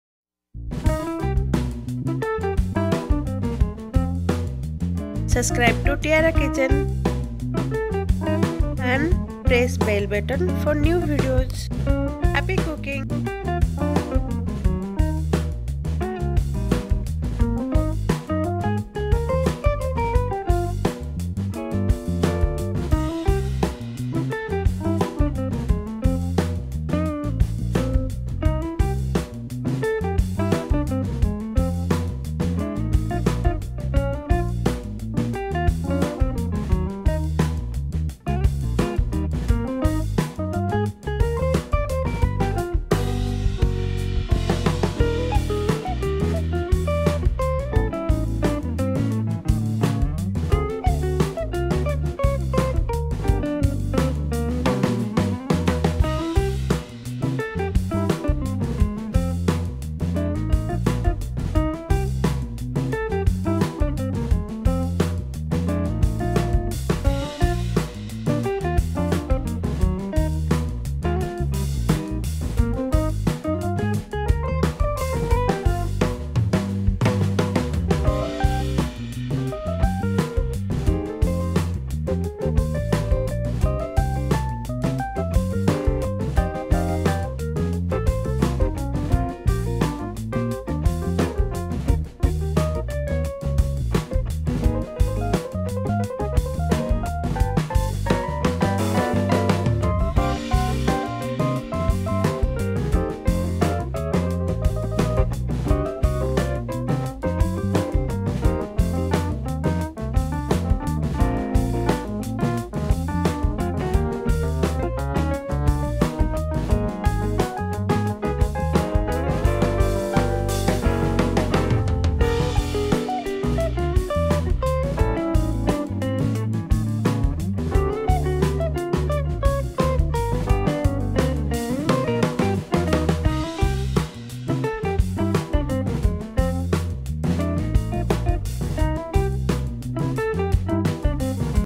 ସଂକ୍ରାନ୍ତି ସ୍ପେସିଆଲ ମକର ଚାଉଳ|makar Chaula|Makar Sankranthi Special|Fasting/vrat Recipe|Makar Rice
As you all know that Sankranti (The Makar Sankranti) is approaching so fast hence Tiara Kitchen has brought something related to Sankranti festival,the famous "Makar Chaula".Its celebrated in every part of our country and we bring this as part of our video program.Its made of dahi chenna (Cheese cottage) and the ingredients which are used as part of our odia tradition and culture.But you can still use other fruits and make it fruit custard also.Its easy to prepare at home and taste is really awesome.Celebrate this Sankranti with something special.With Love Tiara Kitchen.